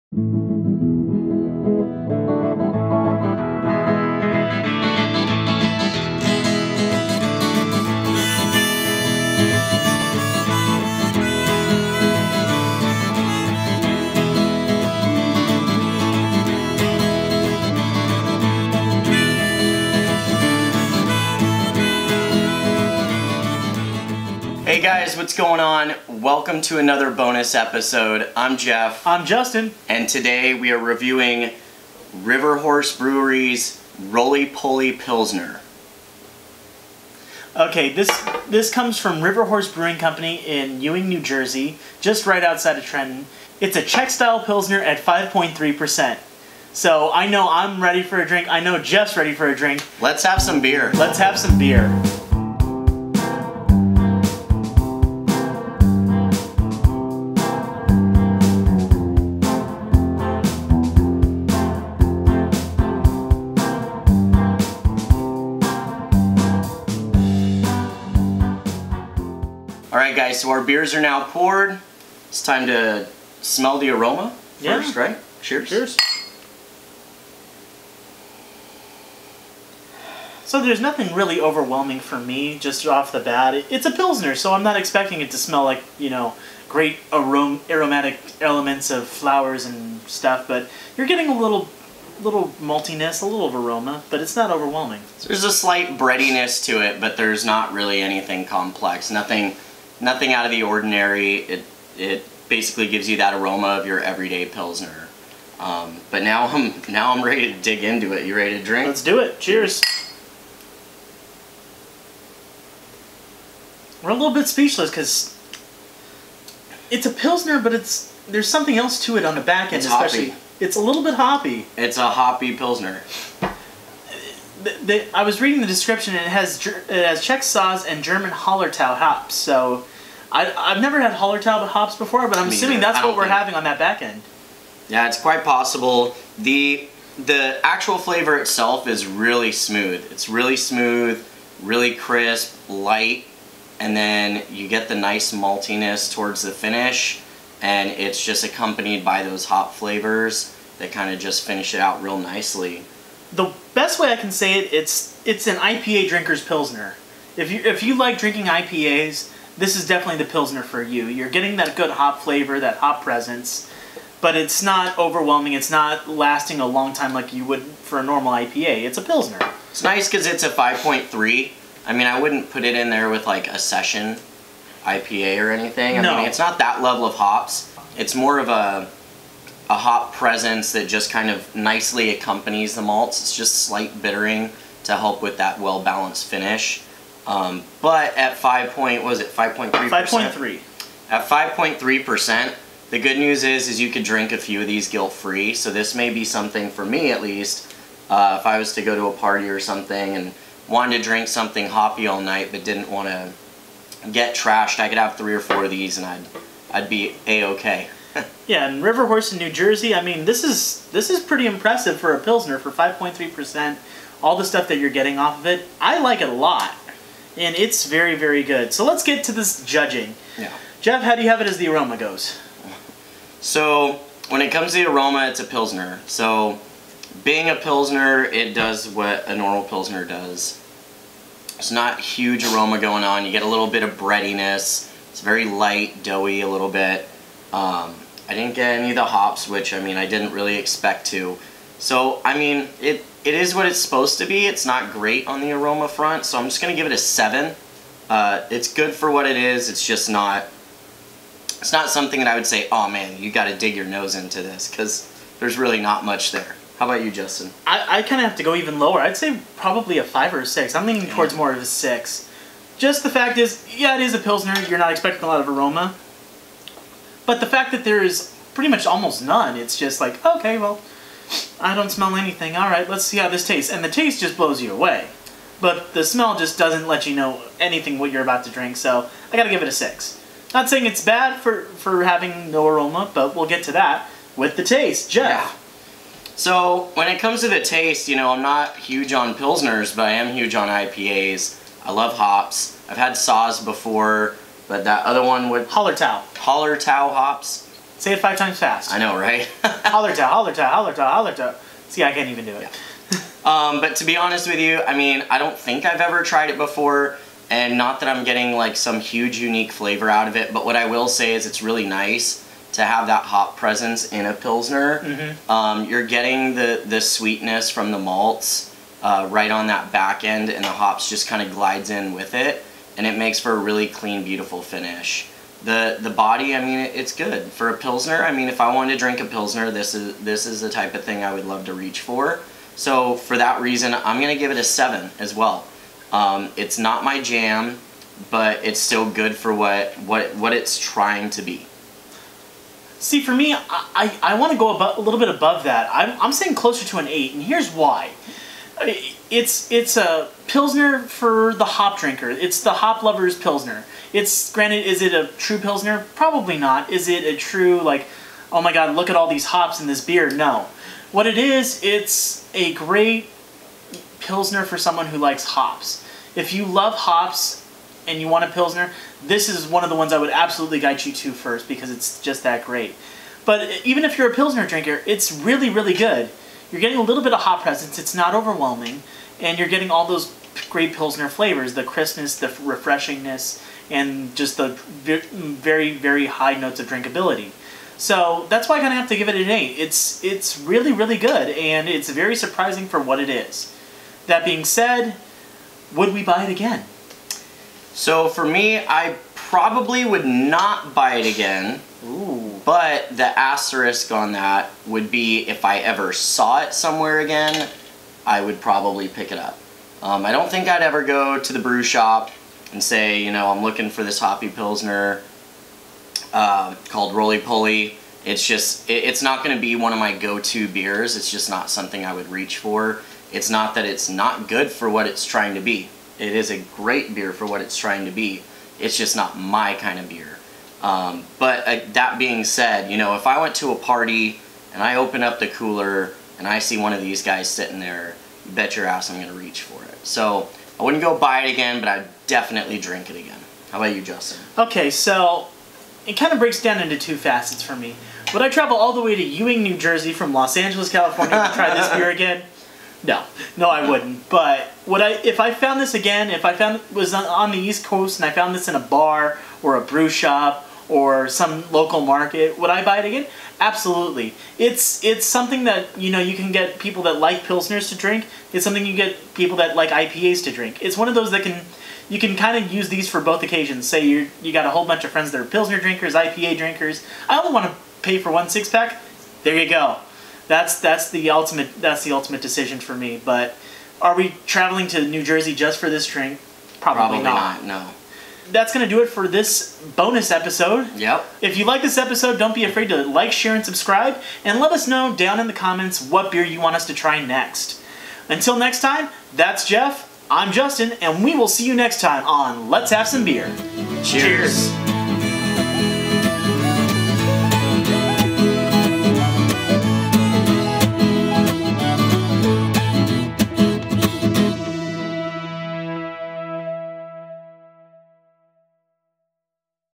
Beer Review: Roly Poly Pilsner (River Horse Brewing) - Let's Have Some Beer Bonus Episode
In this episode, Justin and I review the Roly Roly Pilsner from River Horse Brewing Company. We rate the beer on a scale of 1 to 10 on aroma and flavor and let you know if we would ever drink it again. You don't want to miss this!

Credits to: River Horse Brewing Company in Ewing, NJ